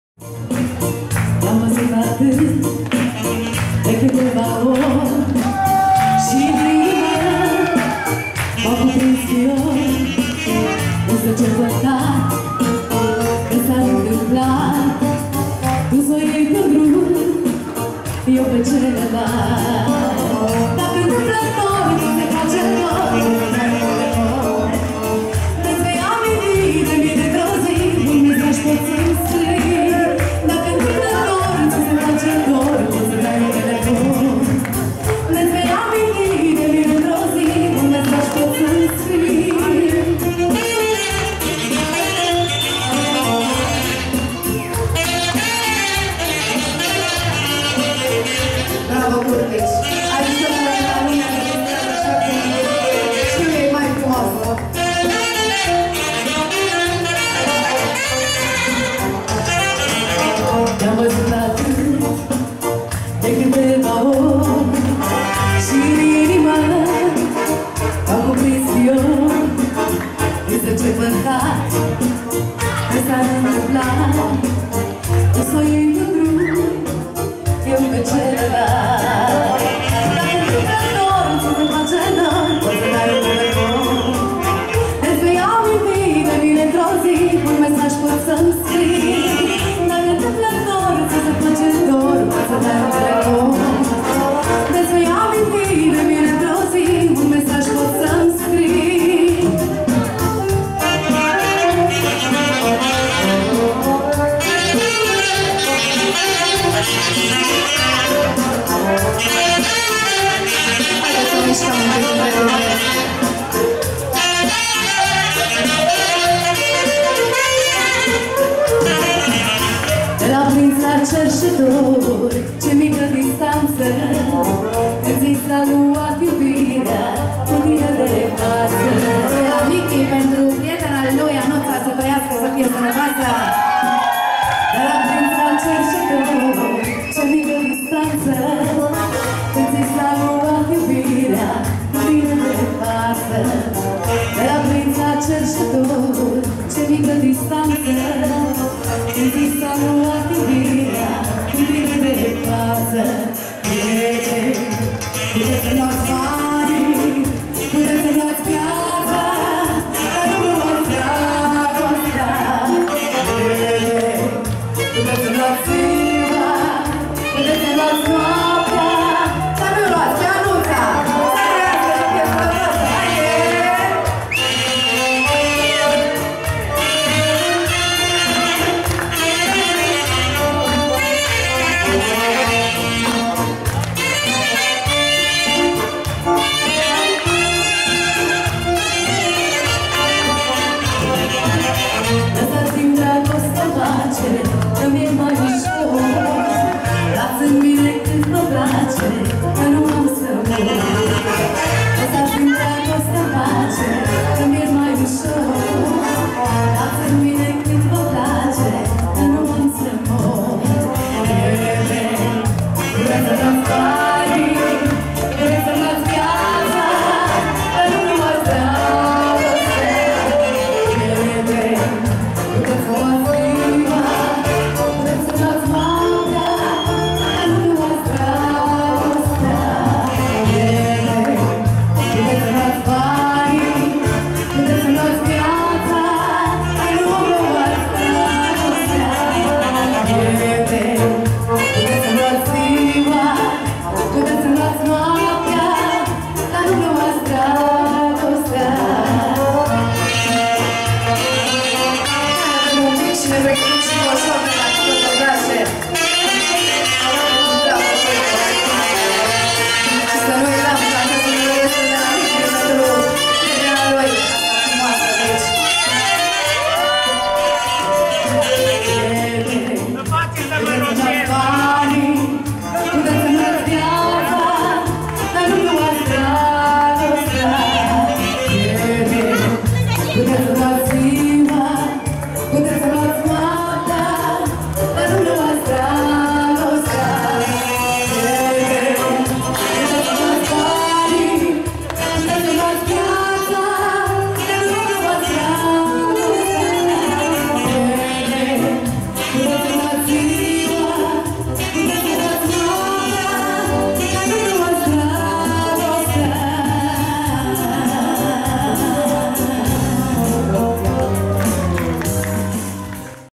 Luciana Giurgiu si formatia Armonia la bal
Zilele orasului Curtici - Mare bal_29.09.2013